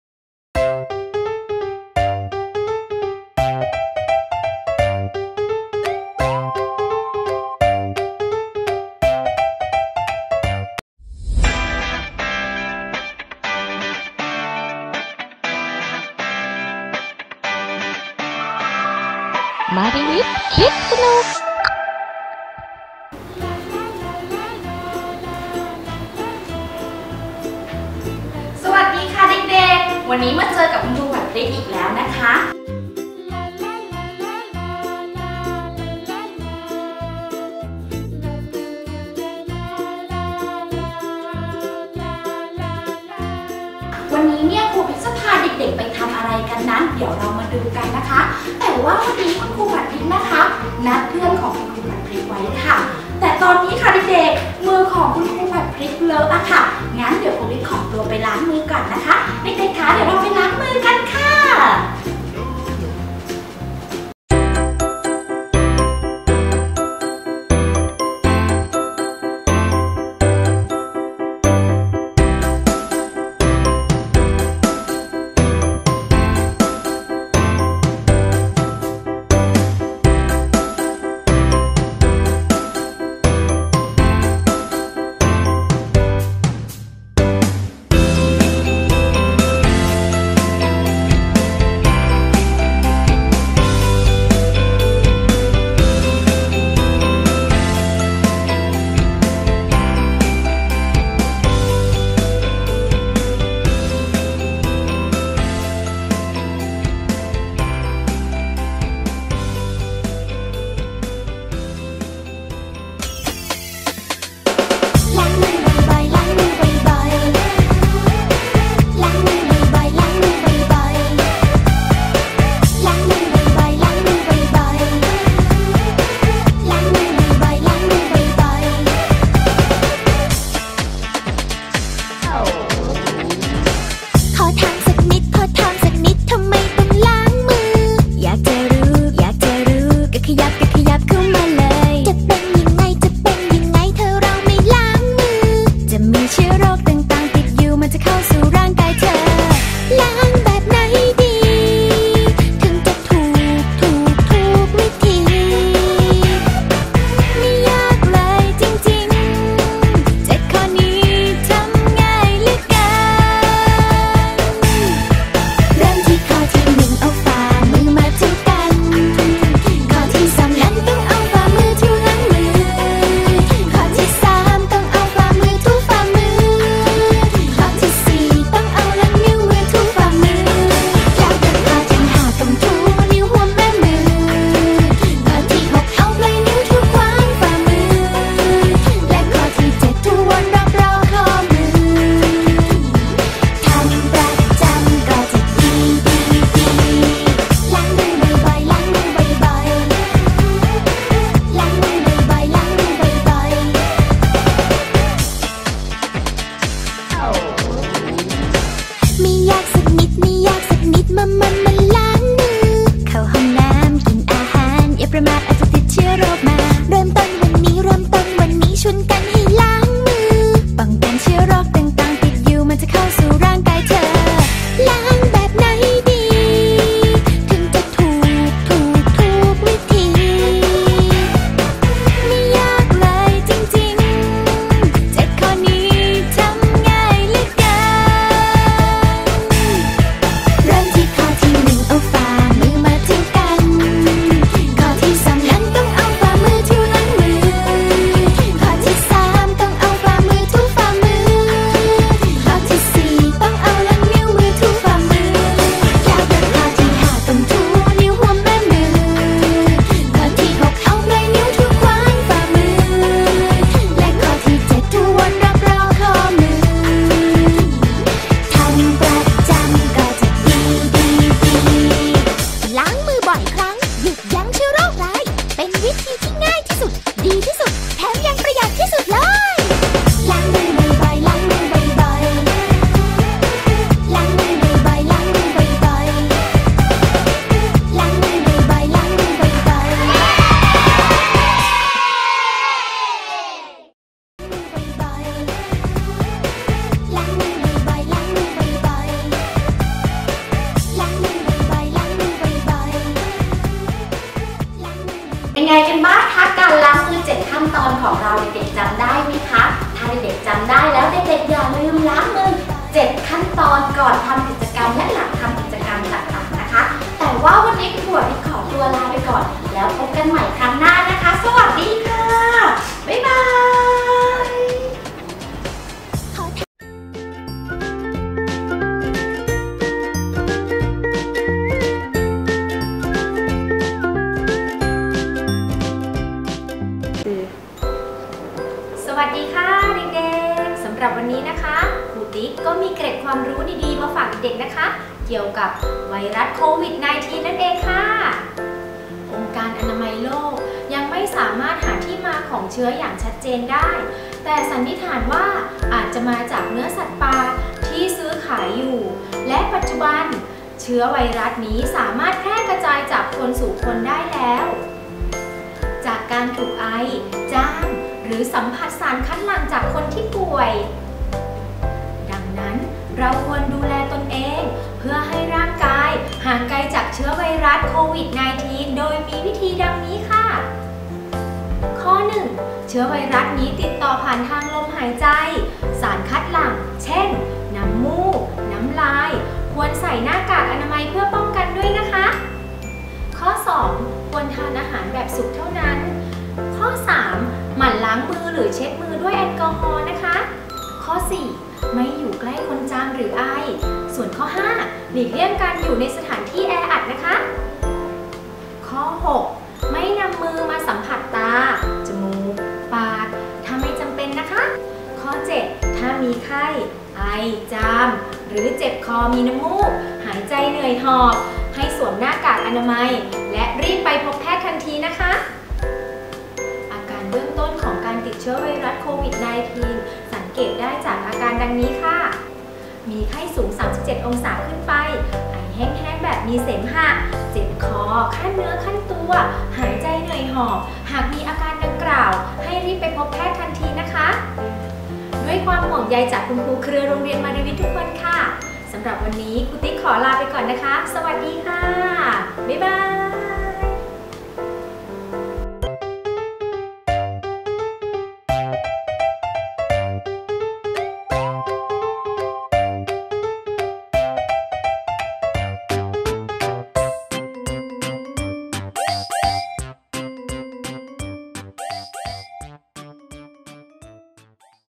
Maryvit Kids สนุก EP.2 | ล้างมือ
📍 Maryvit Kids สนุก 📍 วันนี้จะพาเด็ก ๆ ไปล้างมือ เพื่อป้องกันไวรัส COVID-19  มาร่วมร้องเเละสนุกไปพร้อมกันที่ Maryvit Kids สนุก EP.2 | ล้างมือ